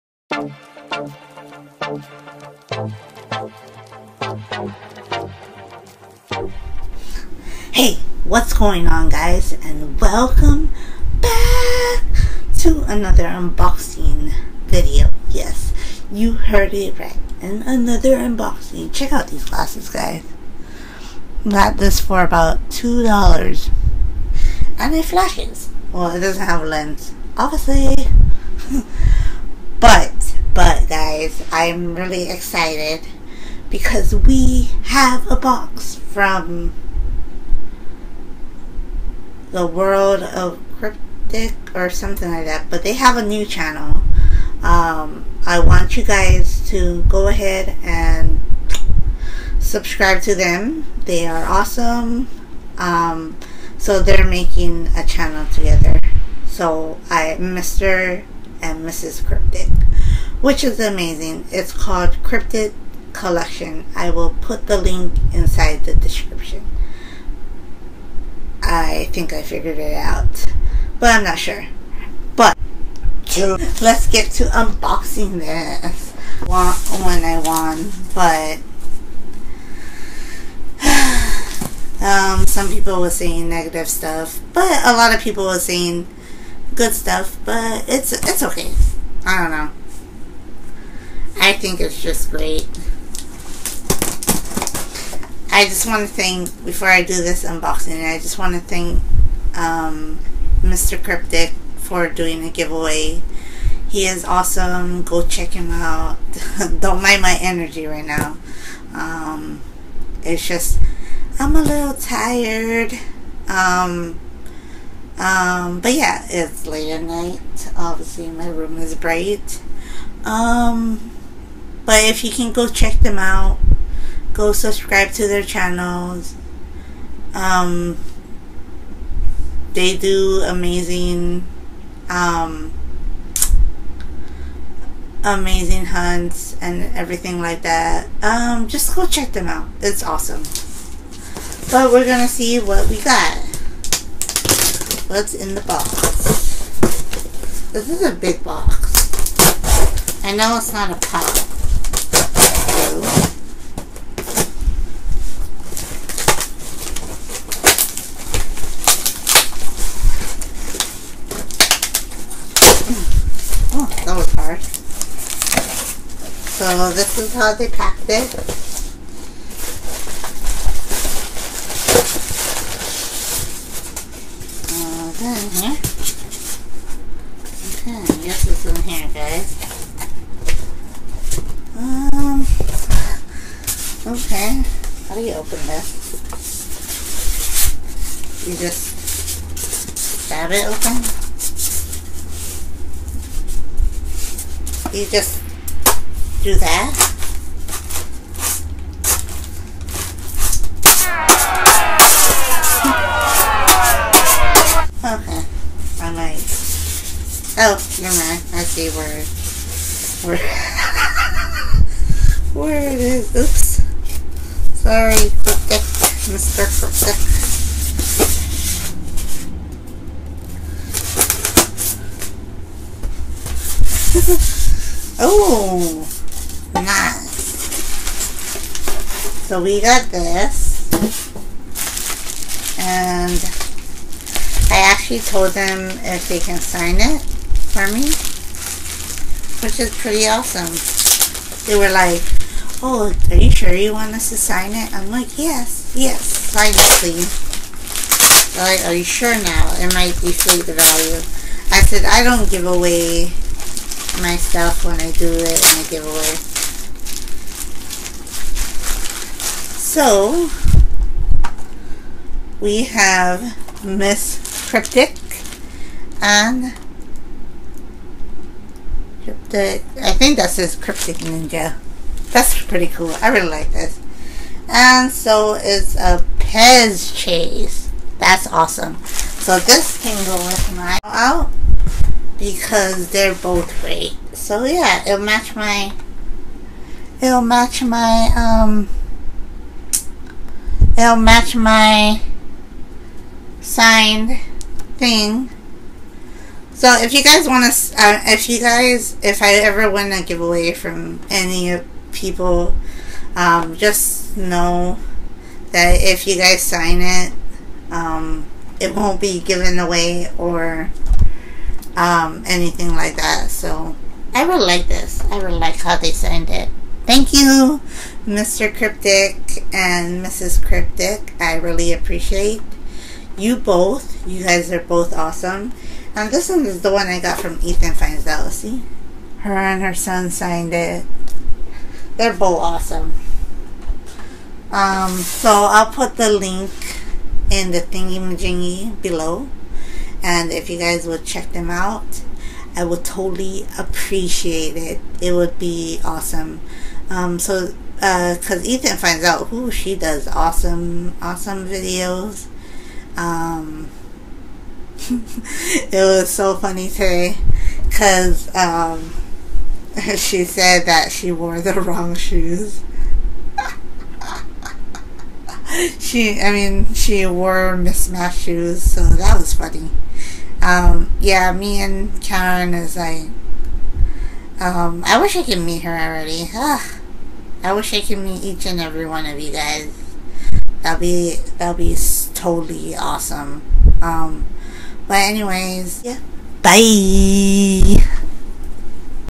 A mail call from kryptic world of funko:)
Hey guys I won a giveaway and I love it 😍 thanks kryptic world of funko I really do appreciate it go subscribe to them if you haven't yet I hope you folks enjoy sorry I'm a little tired 😴 no energy but love yahhhhhhh byeeeeeee